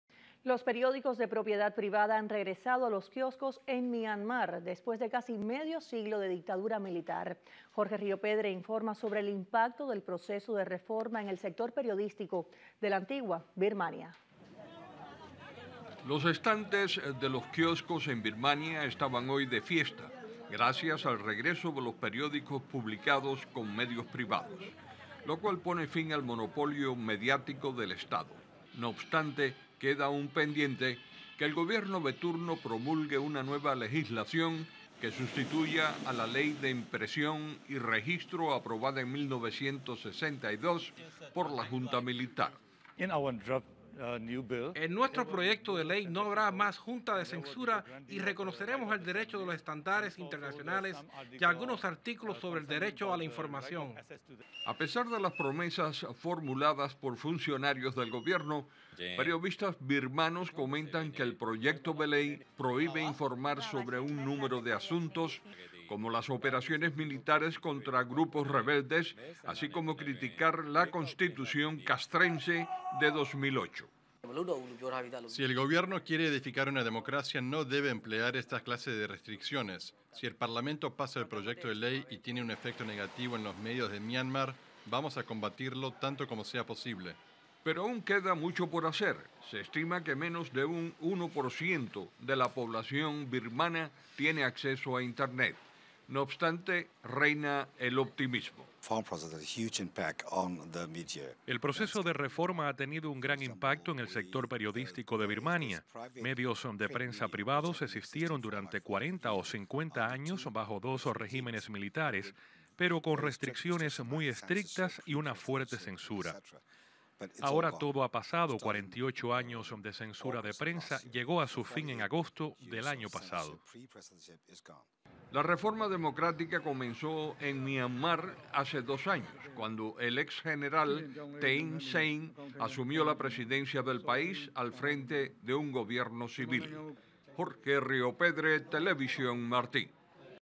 TV Martí Noticias — Renace la prensa libre en Birmania
(TV Martí Noticias) — Los periódicos de propiedad privada han regresado a los quioscos en Myanmar, después de casi medio siglo de dictadura militar. Jorge Riopedre informa sobre el impacto del proceso de reforma en el sector periodístico de la antigua Birmania.